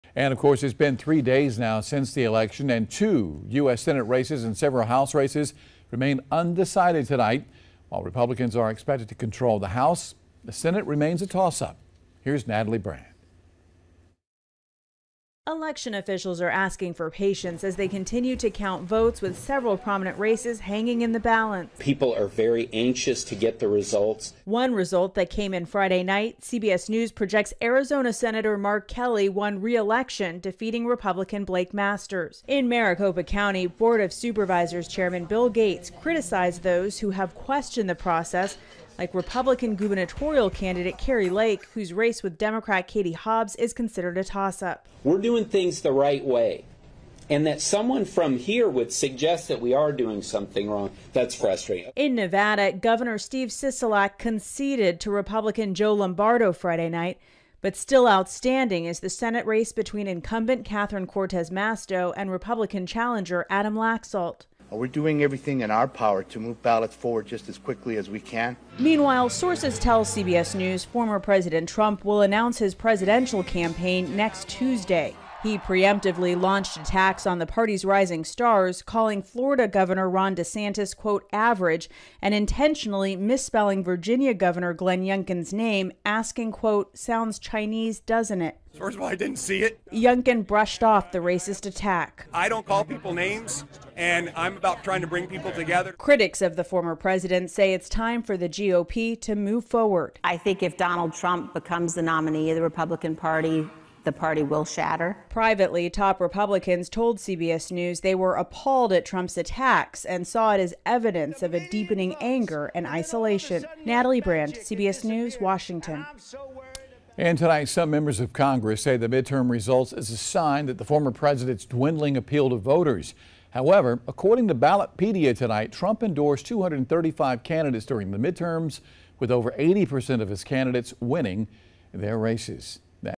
Mark Kelly wins Arizona Senate race, putting Democrats closer to Senate majority
Democrats can retain control of the Senate by winning either the Nevada race, which remains too early to call, or next month’s runoff in Georgia.